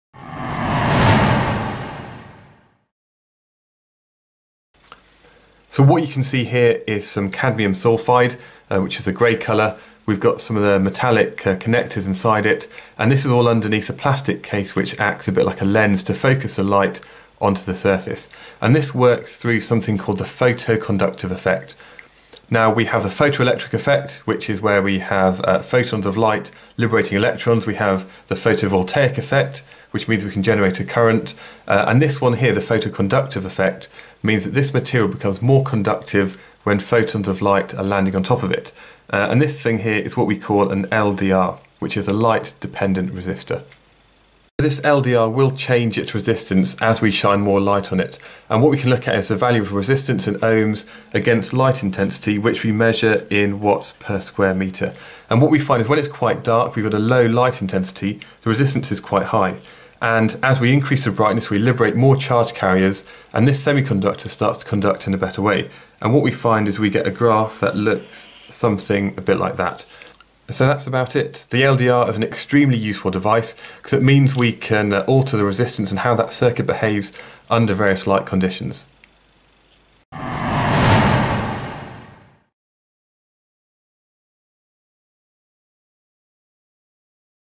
The LDR (Light Dependent Resistor) - A Level Physics
This video introduces and explains the LDR (the Light Dependent Resistor) for A Level Physics.

As the name suggests, an LDR is an electrical component whose resistance is dependent on the light it receives.

Lewis

AQA
CIE
Edexcel
Edexcel IAL
Eduqas
IB
OCR A
OCR B
WJEC